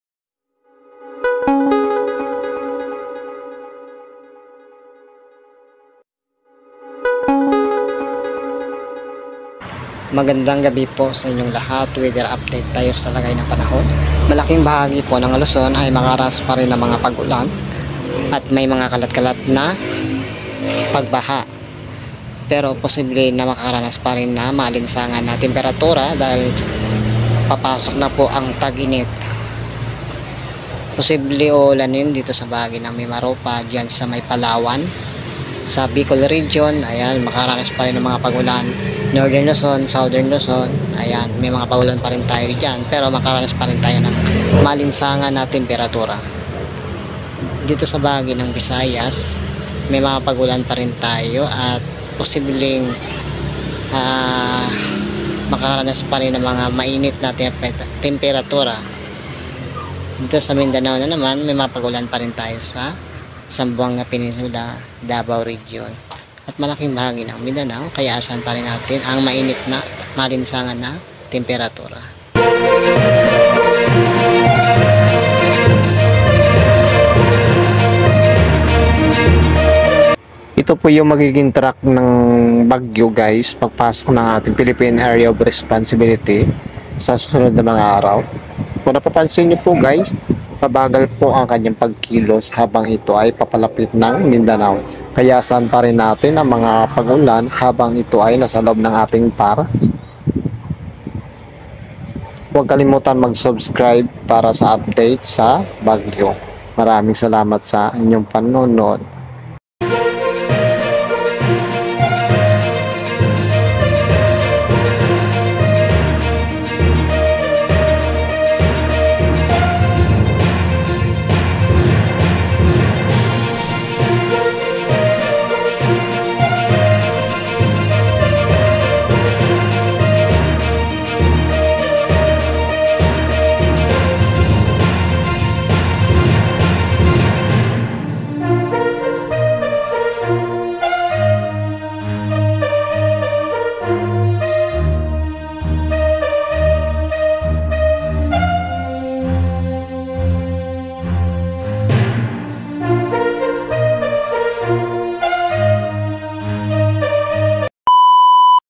BAGYO SA LABAS NG PAR. PAPASOK NASA PILIPINAS
Isang LPA sa labas ng PAR Posible lumakas pa at pumasok sa loob ng Pilipinas.